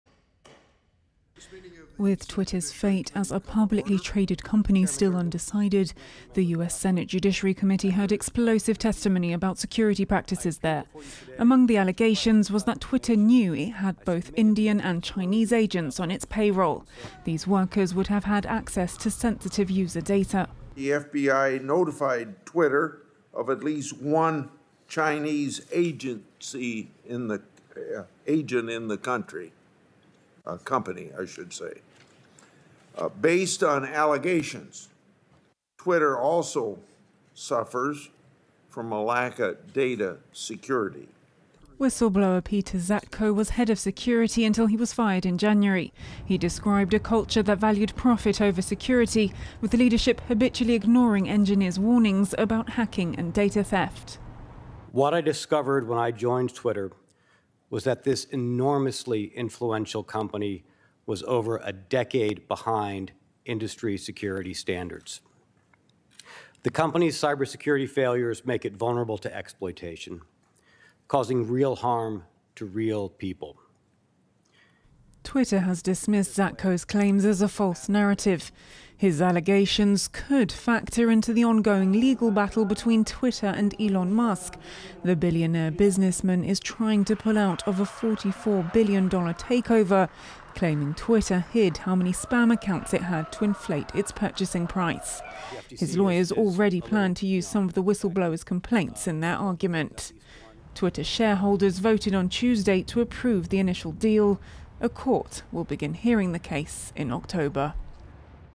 Twitter whistleblower reveals concerns over China gathering users data • FRANCE 24 English
The FBI informed Twitter Inc. of at least one Chinese agent working at the company, U.S. Senator Chuck Grassley said during a Senate hearing on Tuesday where a whistleblower testified, raising new concerns about foreign meddling at th influential social media platform.